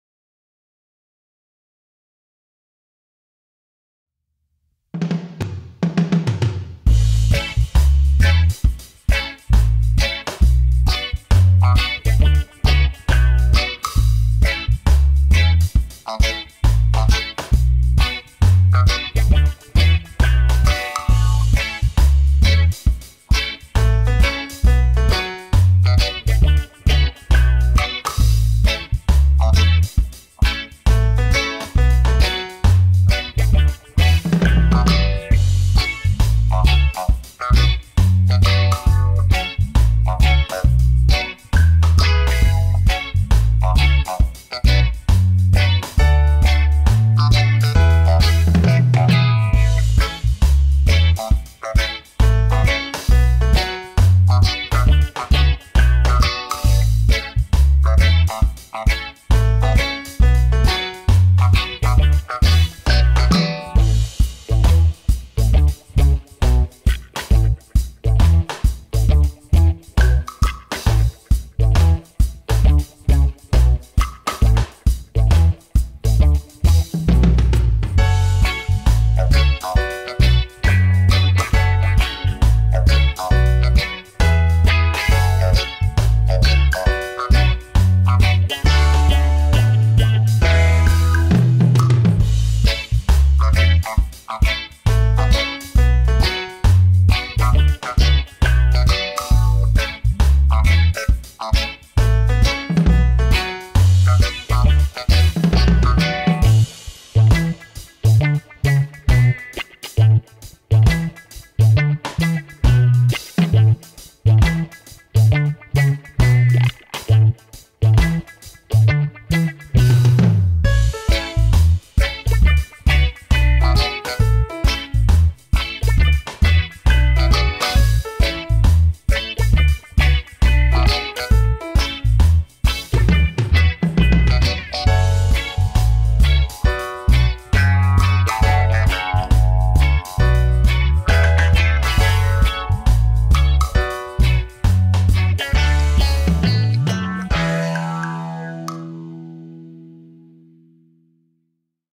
如何畫SPY×FAMILY大白狗彭德·佛傑,超簡單加陰影｜How to Draw Bond Forger(ボンド・フォージャー) [ Timelapse ]｜康色 Kang sir
最初因一些原因落入恐怖組織手中，當作狗體炸彈使用，與安妮亞相遇後，與安妮亞一同阻止了爆炸事件。事件結束後被佛傑家收養，成為家族的一員。

有預知未來能力的大型犬。
佛傑家的寵物。可以背著安妮亞行動。
名字從安妮亞喜歡的間諜動畫《SPYWARS》的主角－彭德曼命名。

0:00 頭和身體 Head and Body
0:34 下半身和腳腳 lower body
0:50 臉 Face
1:03 陰影+著色 Shadowing and Coloring
2:22 完成囉 Well done!

大家追起來!😘😘